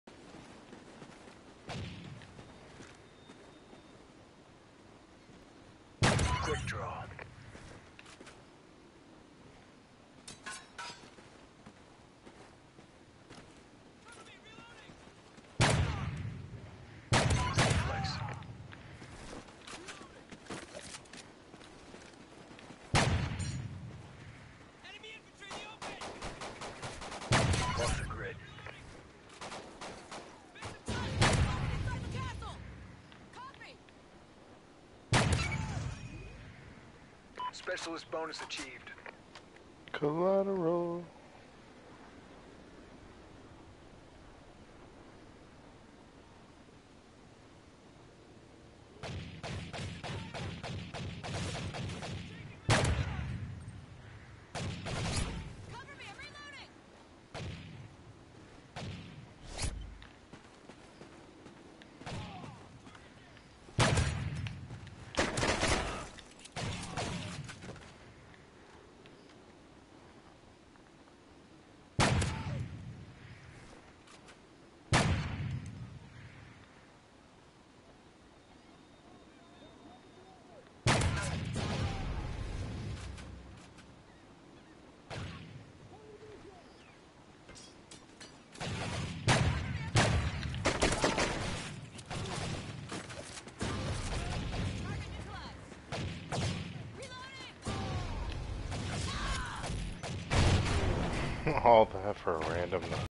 Stonehaven Collateral rocking until Random Knife.
Was 13 and 0 and then a random knife toss ruined it all.